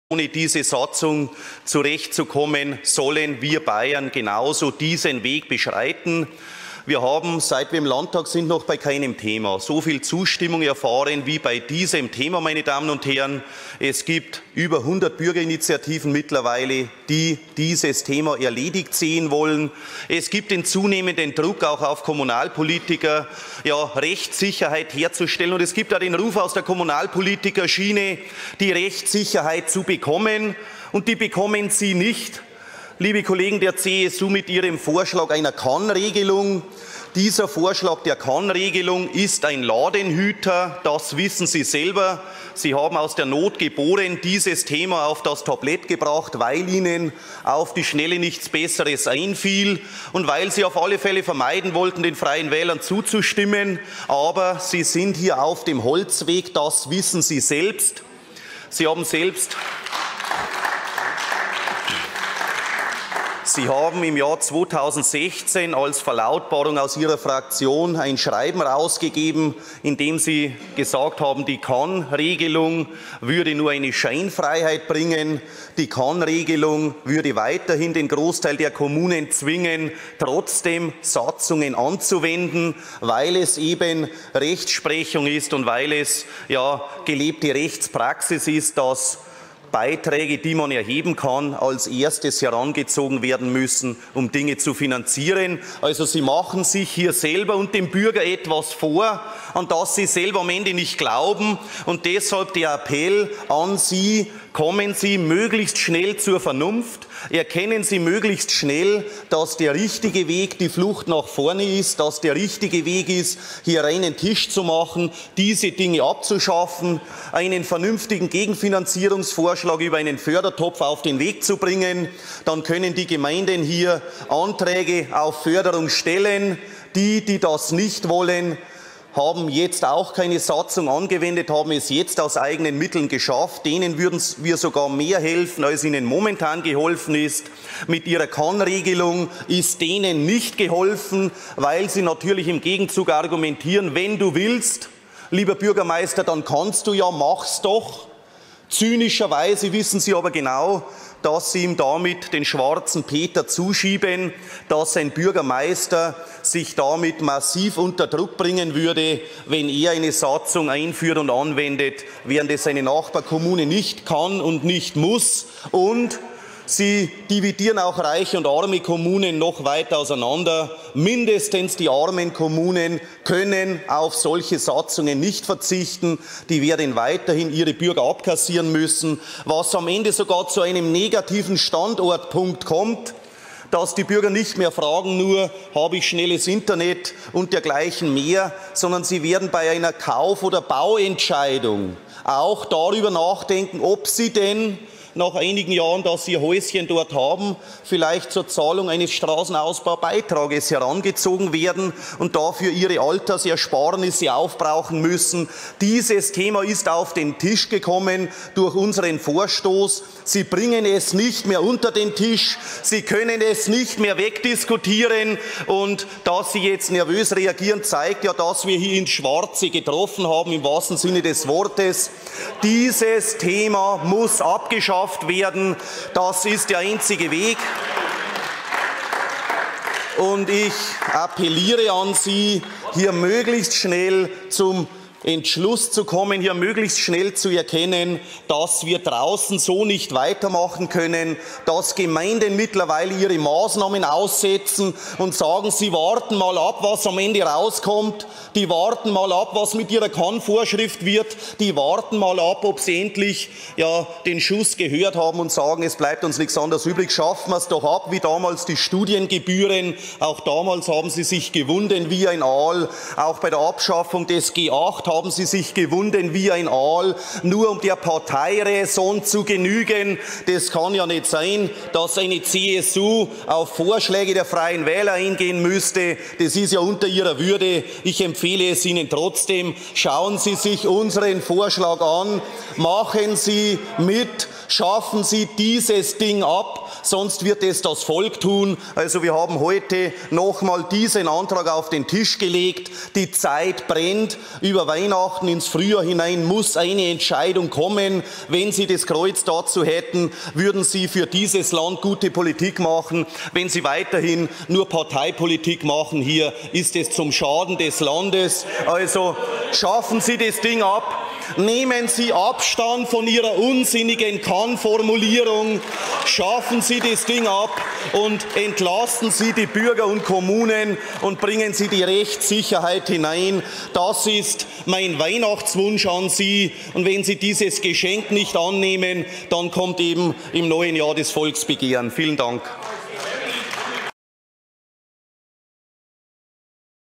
Strabs abschaffen – Volksbegehren steht unmittelbar bevor - Hubert Aiwanger FREIE WÄHLER
Wir FREIE WÄHLER wollen die Straßenausbaubeiträge in Bayern abschaffen – das hat unser Fraktionsvorsitzender Hubert Aiwanger in seiner Plenarrede noch einmal klargestellt.